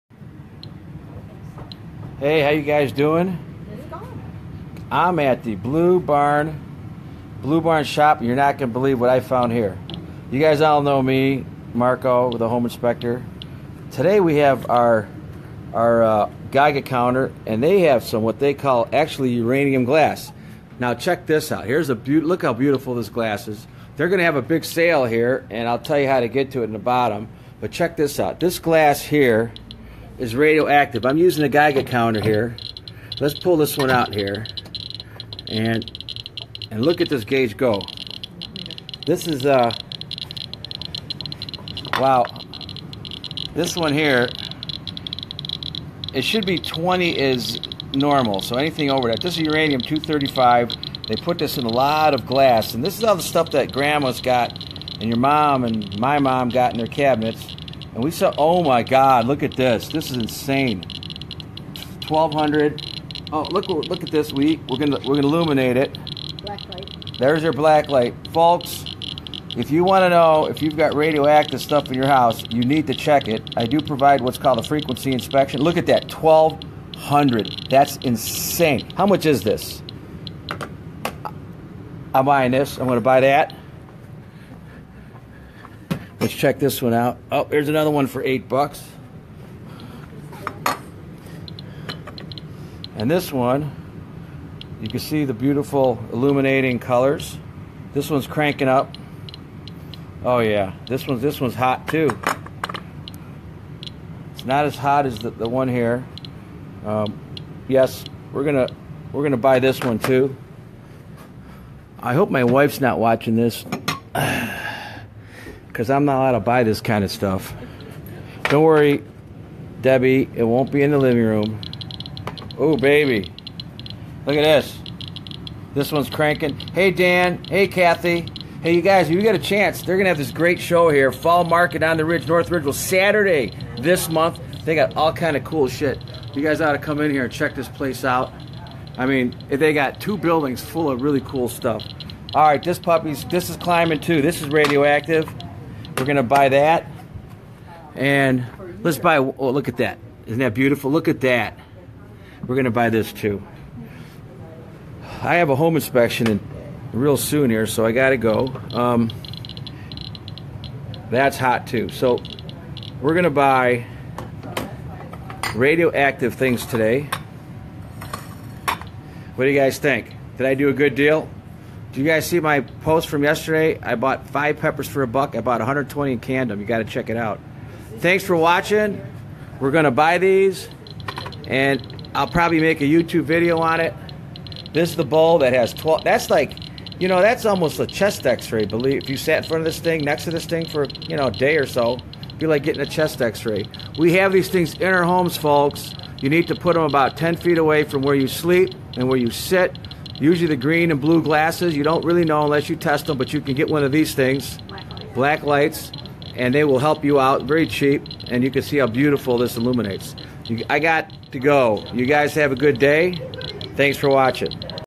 Uranium Glass: Your house decorations may be radioactive, Green Glass
Your house decorations may be radioactive. Uranium was used in green glass.